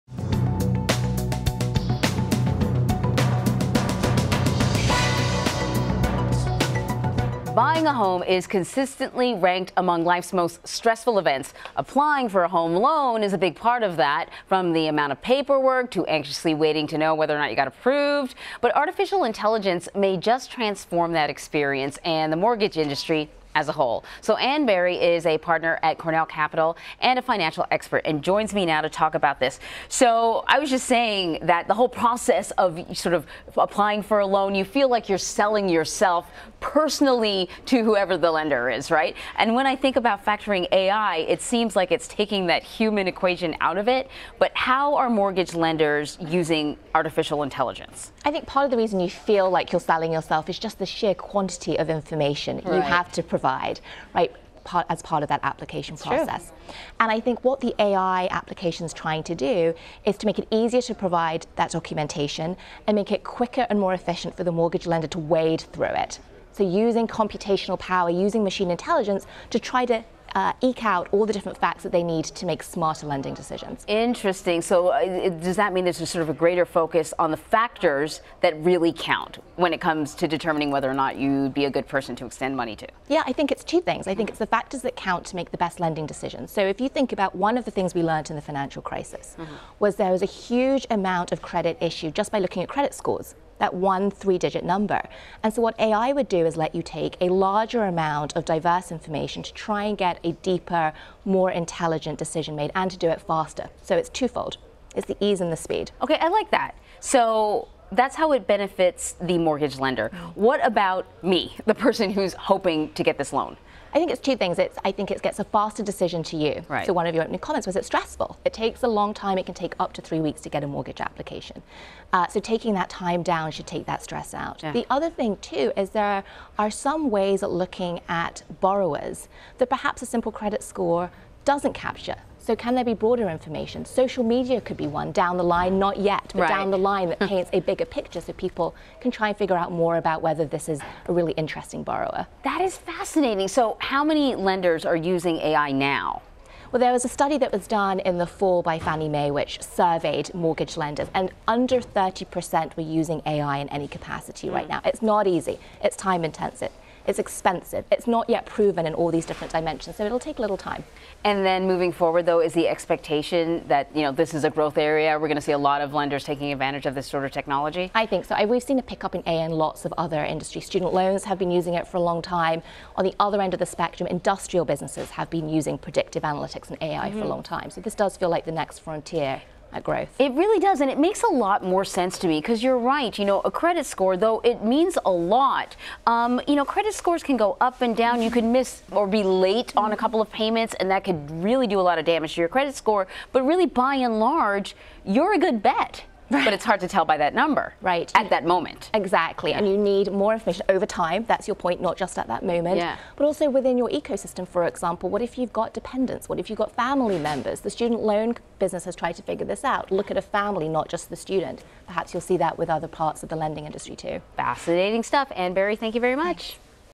How artificial intelligence is transforming the mortgage industry
Financial expert Ann Berry explains how artificial intelligence is transforming the mortgage industry for lenders and loan applicants.